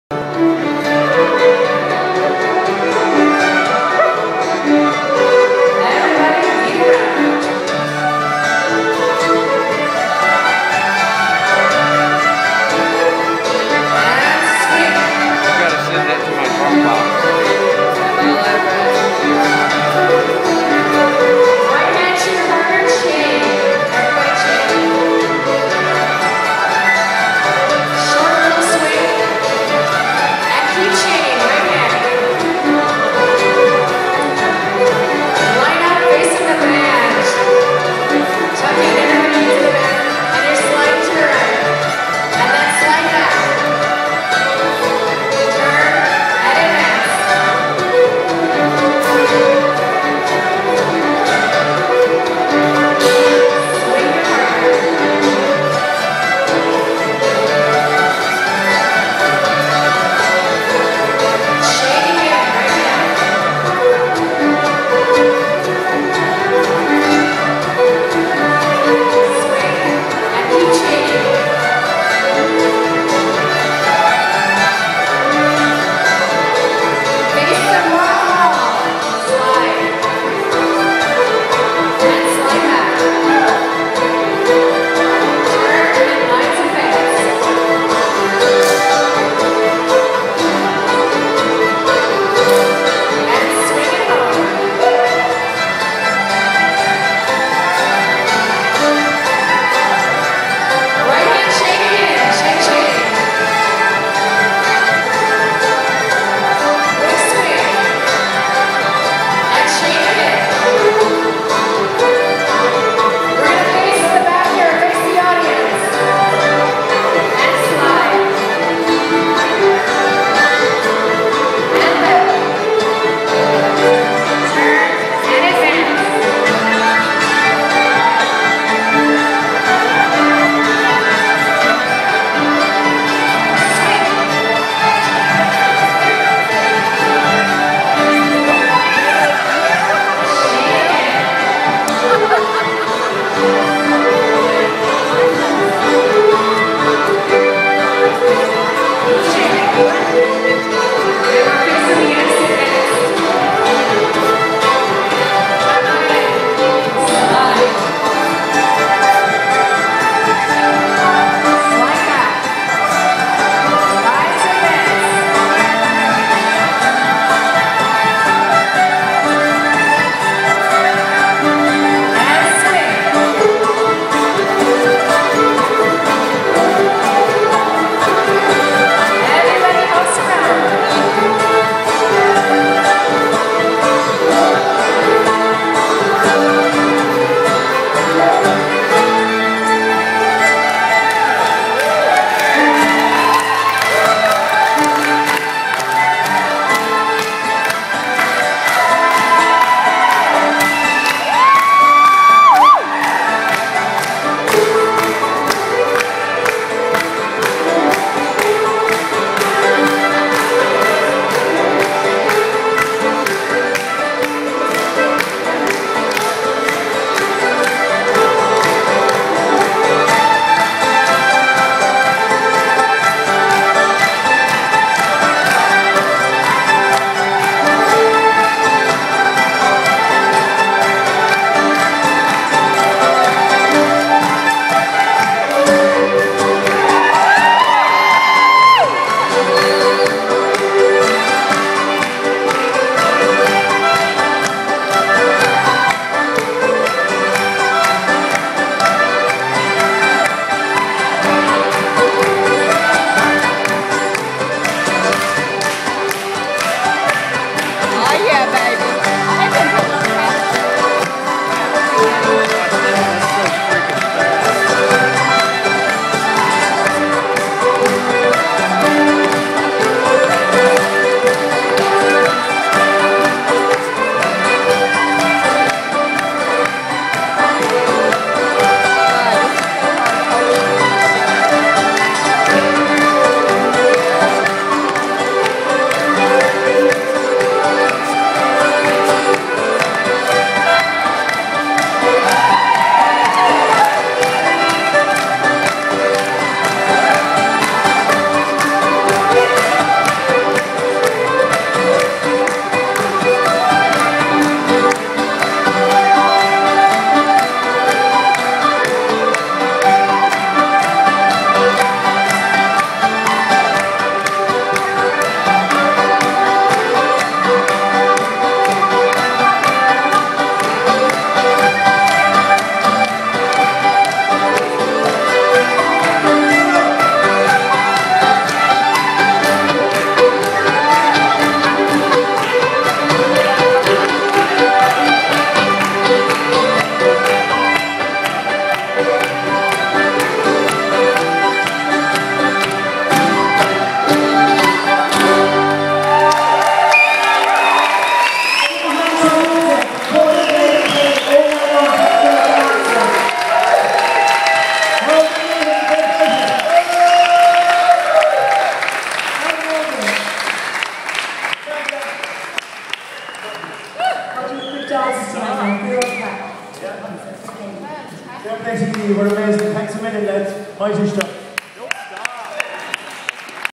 All-Star Ceili: The Swannanoa Gathering, 2013
Shot at the Swannanoa Gathering: Celtic Week, this video features Martin Hayes, Kevin Crawford, Kathleen Conneely, Grainne Hambly, Marla Fibish, John Whelan, Brian Conway and Eamon O'Leary, and is called by Maldon Meehan.